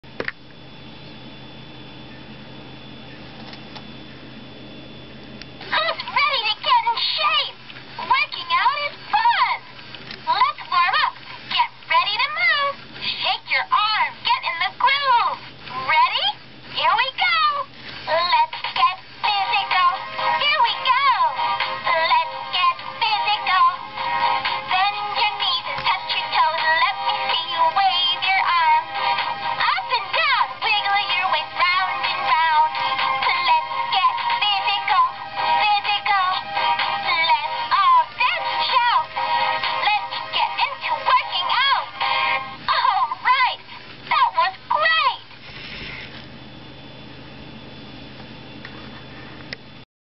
eBay Item Demo - Exercising Care Bears Harmony Bear
Animatronic Fun and Fit Harmony Bear about to go up for sale on eBay